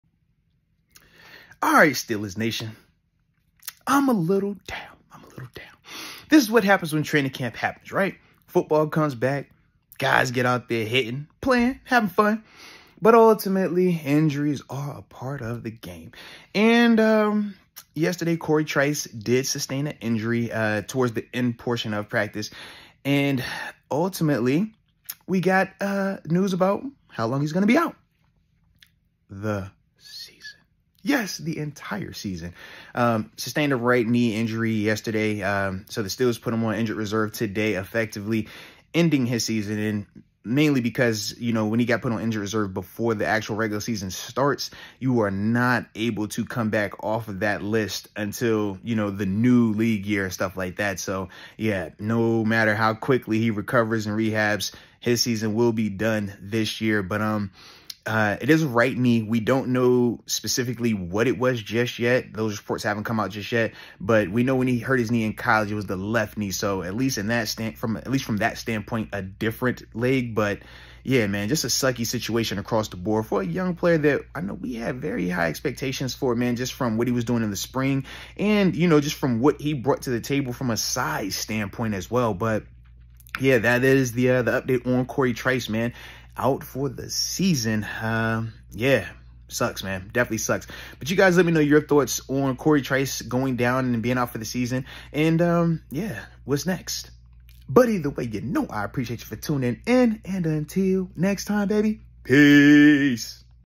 Pittsburgh Steelers Cory Trice Placed On Injured Reserve For The Season!
Arthur reacts to the Steelers placing Cory on IR!